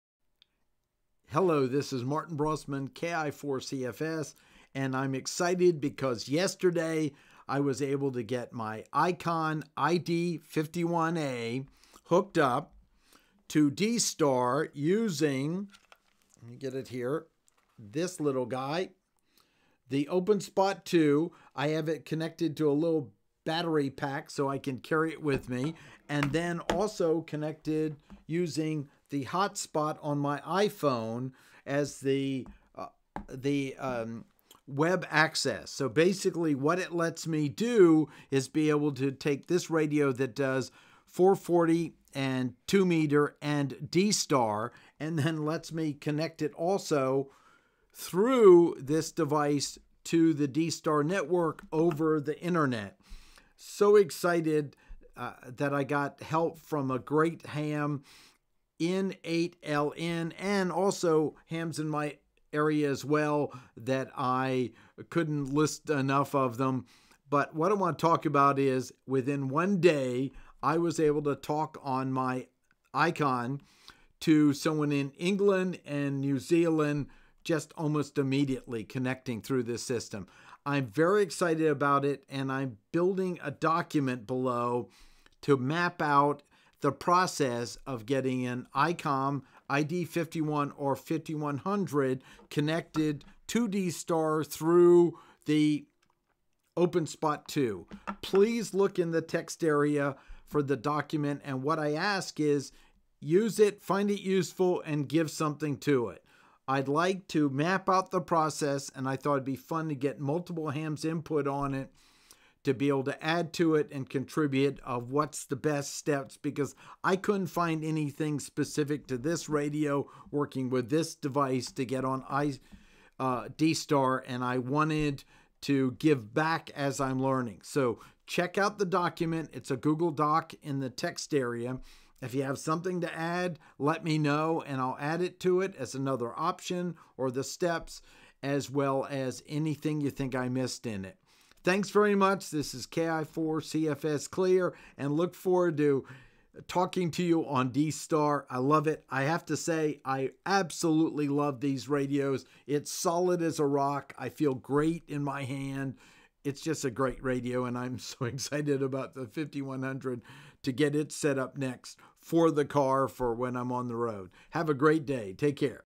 Setting up D-Star to work off an iPhone hotspot using the openSPOT2, an ICOM 5100A and ICOM 51A
Here is my Google Docs on Setting up D-Star to work off an iPhone hotspot using the openSPOT2, an ICOM 5100A, ICOM 51A

Thanks to Terry Tassos N8LN for his help!